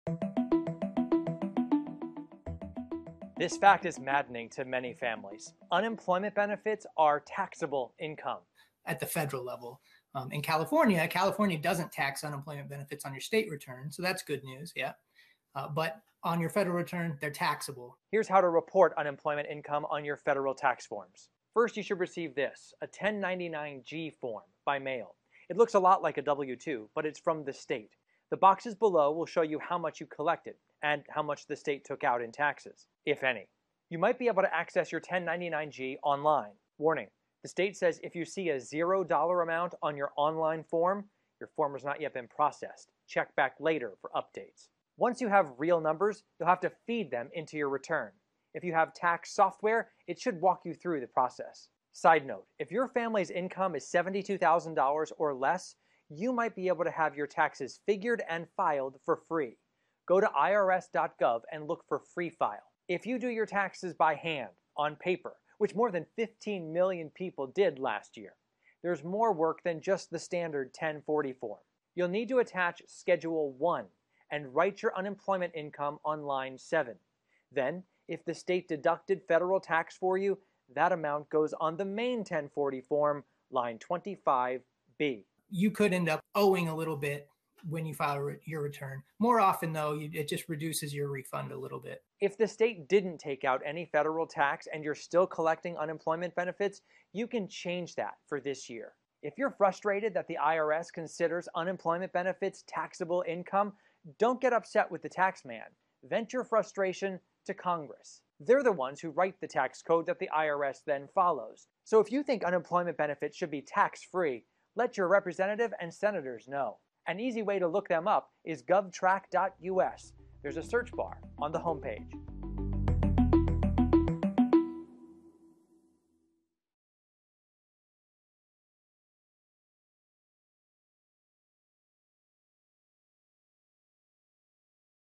Explained: How To Report Unemployment on Taxes
If you collected an unemployment check during the pandemic, you’ll likely have an extra step when doing your federal taxes. Consumer Investigator Chris Chmura shows us “How To.”